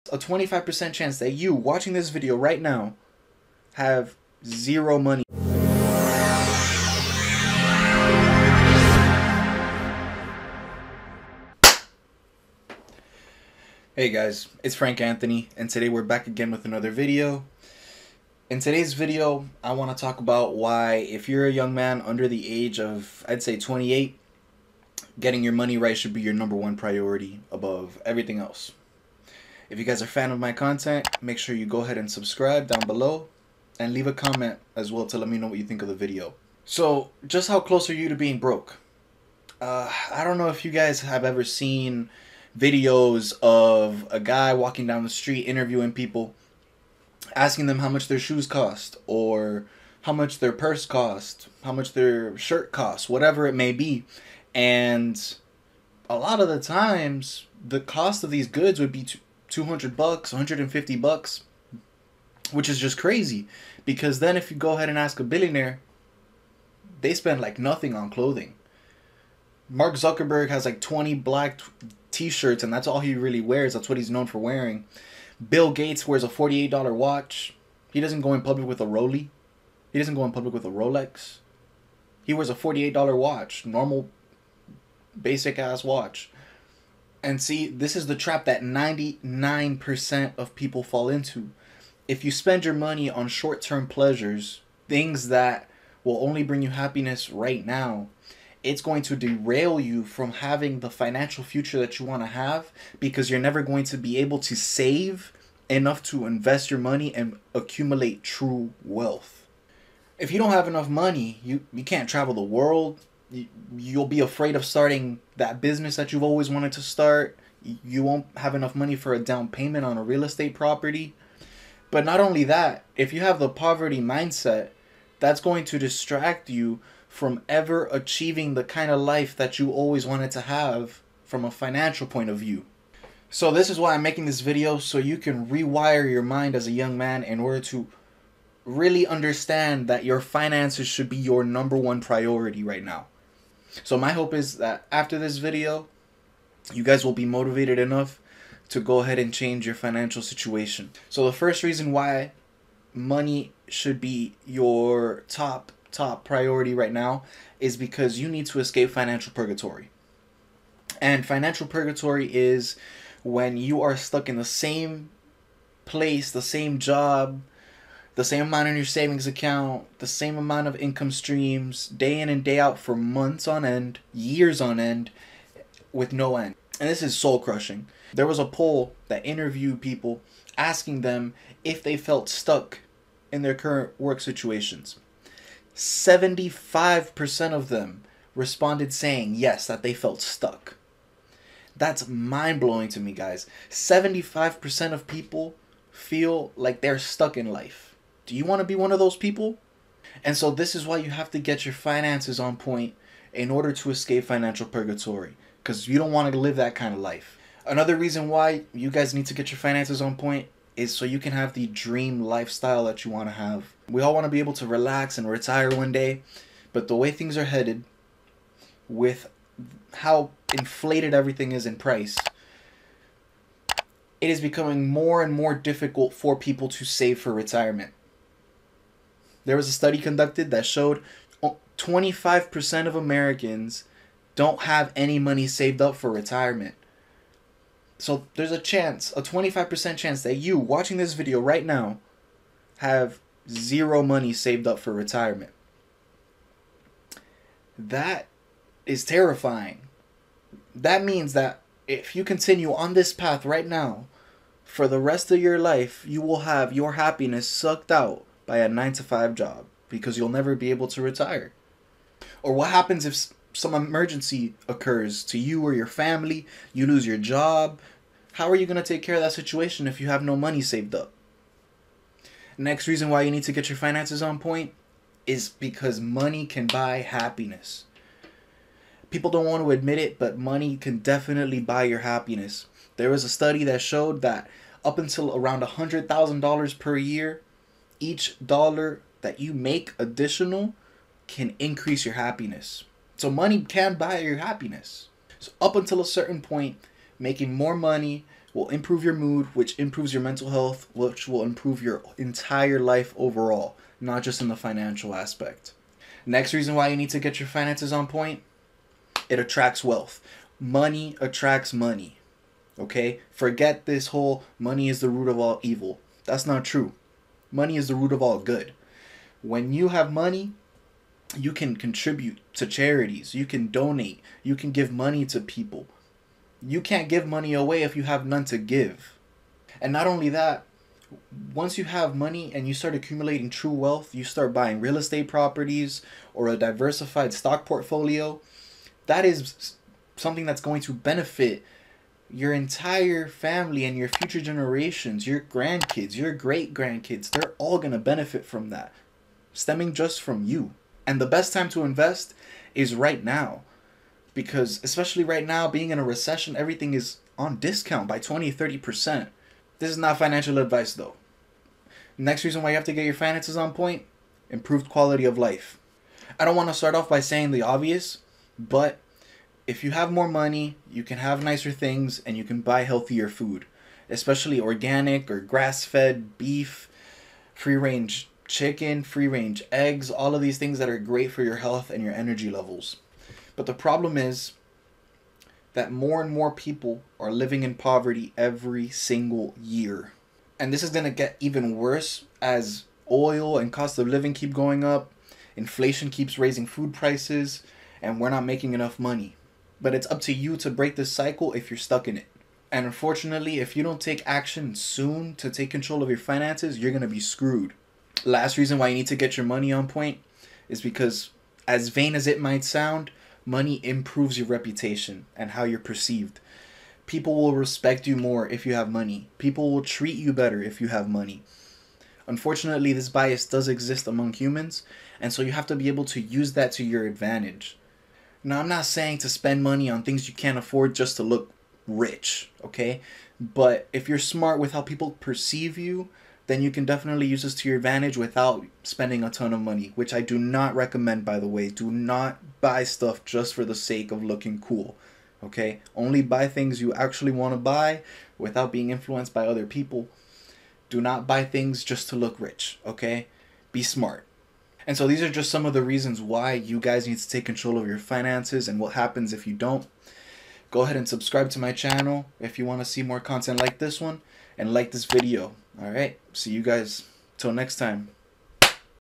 Why Men MUST Have Money
In this video, I'm going to be going over some of the reasons why you MUST prioritize getting your money on point as early as possible.

Having enough money will fix many of your problems as a man, whether you like to admit it or not.

Click the link below to sign up for Robinhood and get a free stock with your first deposit!

Welcome to the official Nurse Frank Anthony youtube channel!

🎬 SUGGESTED VIDEOS